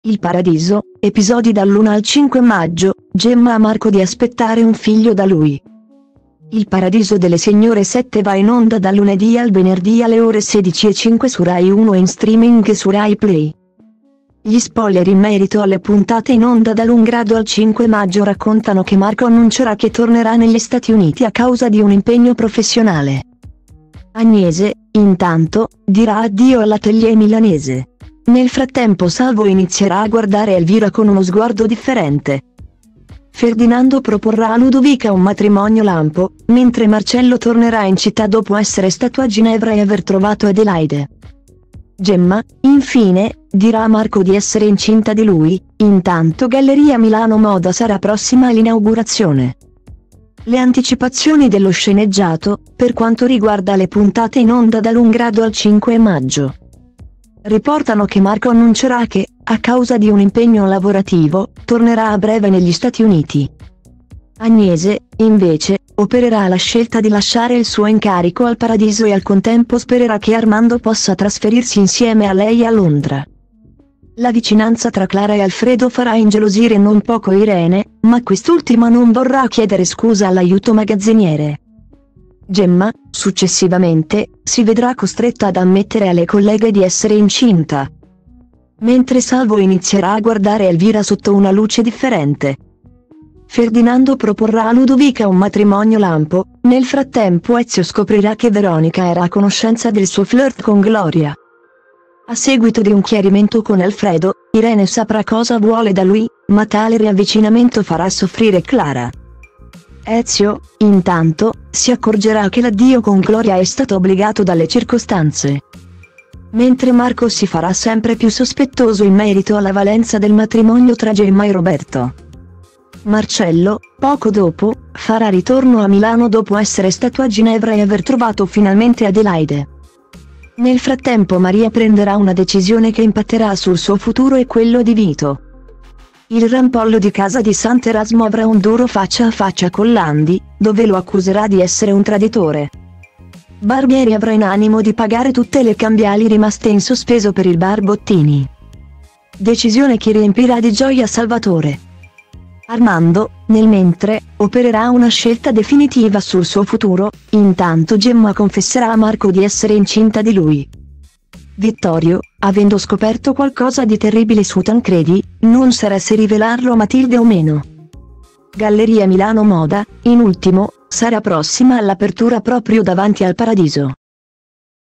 Il Paradiso, episodi dall'1 al 5 maggio: Gemma a Marco di aspettare un figlio da lui
Il Paradiso delle Signore 7 va in onda dal lunedì al venerdì alle ore 16:05 su Rai 1 e in streaming su RaiPlay.Gli spoiler in merito alle puntate in onda dal 1° al 5 maggio raccontano che Marco annuncerà che tornerà negli Stati Uniti a causa di un impegno professionale.Agnese, intanto, dirà addio all'atelier milanese. Nel frattempo Salvo inizierà a guardare Elvira con uno sguardo differente.